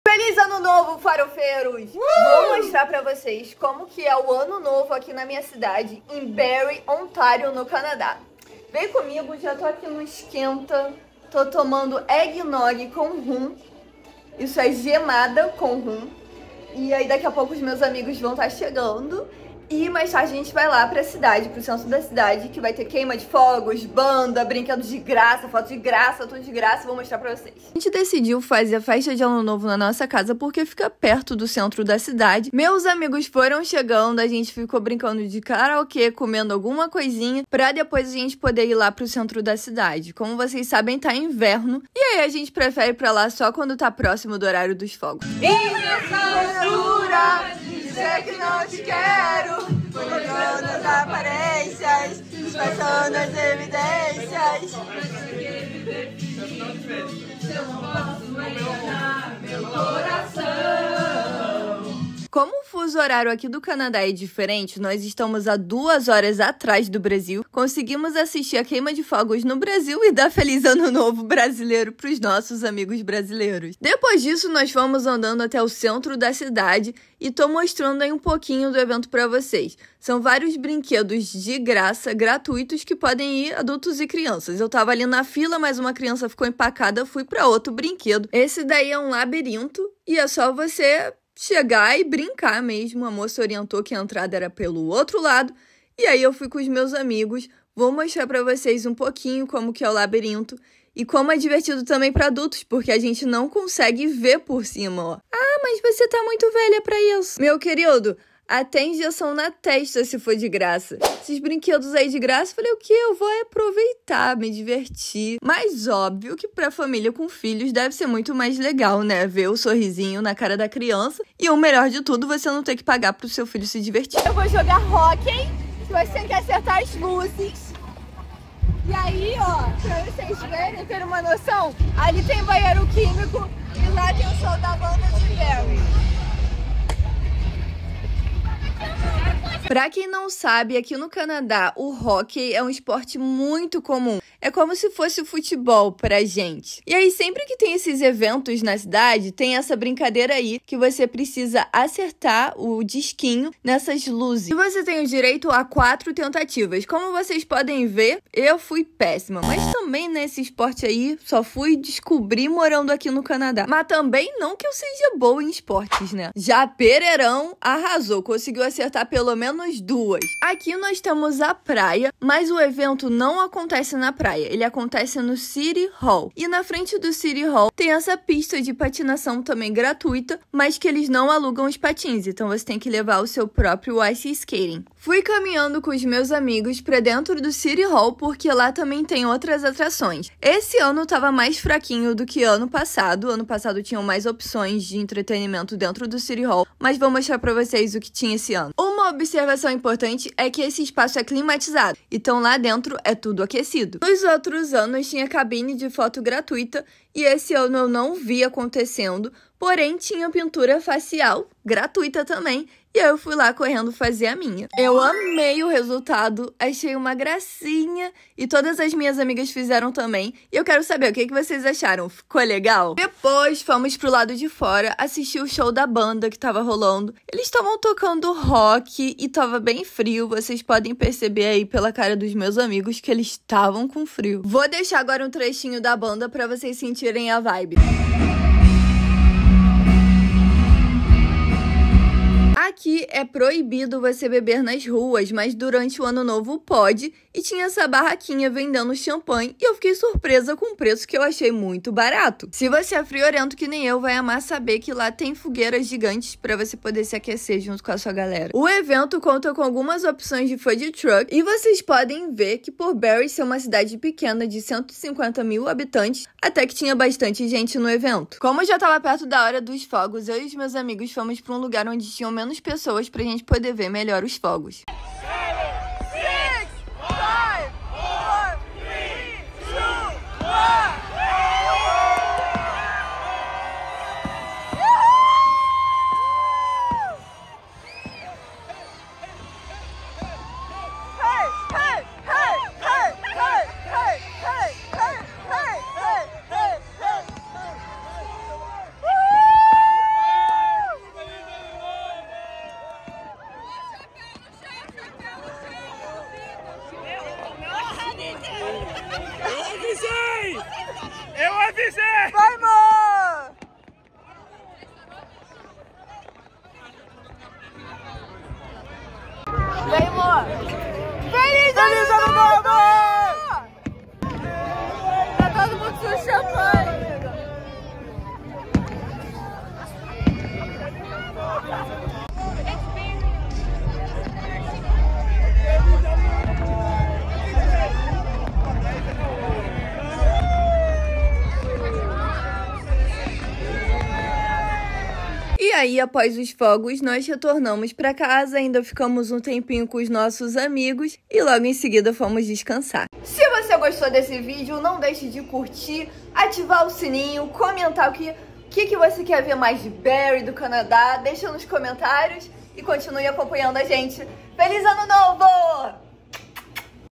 FESTA DE RÉVEILLON NO CANADÁ - Barrie, Ontario
ANO NOVO EM BARRIE ONTARIO Um pouquinho de como foi a festa aqui na cidade que moramos, Barrie - Ontário!

Chegamos no Canadá em dezembro de 2019 e eu ia amar saber como é celebrado essa festa aqui então espero que esse vídeo ajude quem estiver vindo!

Lembrando que eles também fazem queima de fogos 8PM para criançada.

Comenta o que achou! 👇🏽